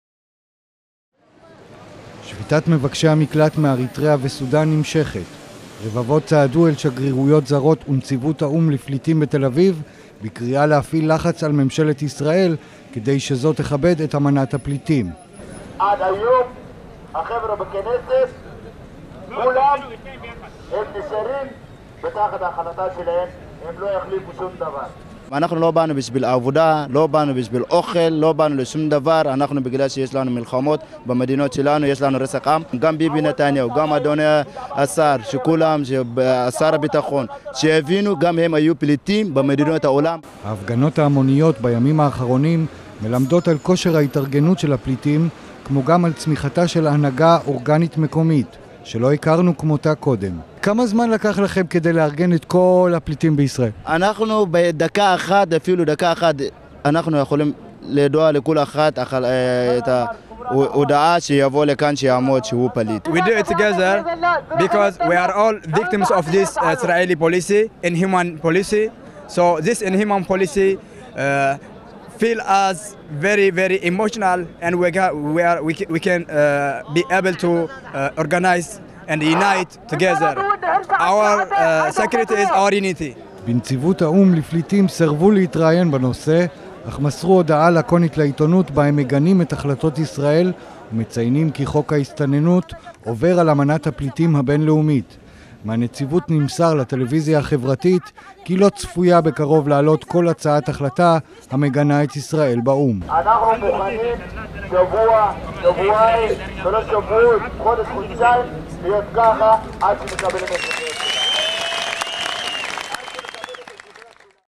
צועדים לנציבות האו"ם - כתוביות
שביתת מבקשי המקלט מאריתראה וסודאן נמשכת- רבים מהם צעדו היום (6-1-2014) אל שגרירויות זרות ונציבות האו"ם לפליטים בתל אביב, בקריאה להפעיל לחץ על ממשלת ישראל, כדי שזו תכבד את אמנת הפליטים.